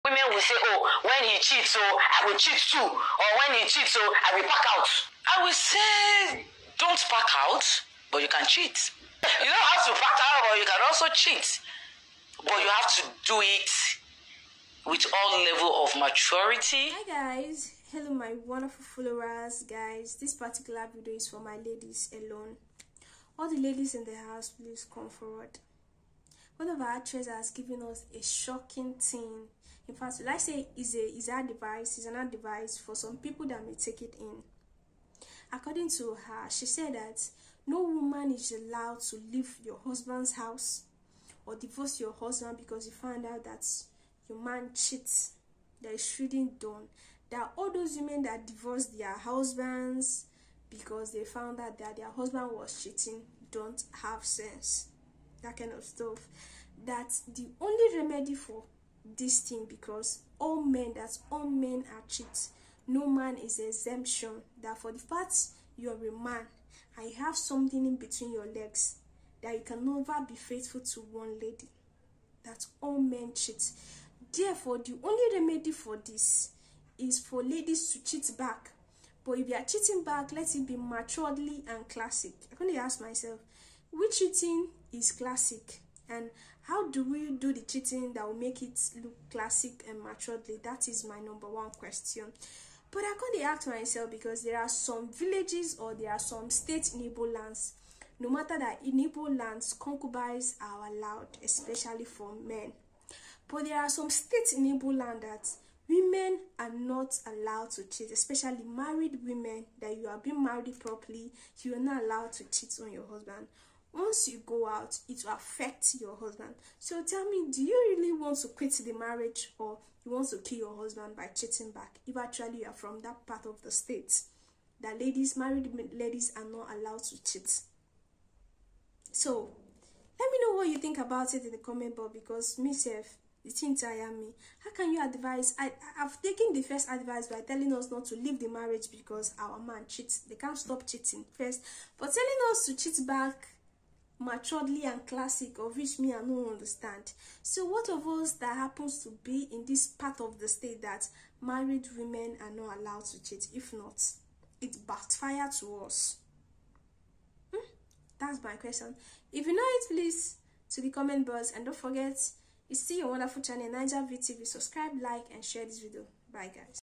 Nollywood Actress Vows To Cheat Back On Cheating Partner
Naija actress Laide Bakare has advised that the only solution to a chatting partner is to cheat back. Our girl Ruthy looks into the issue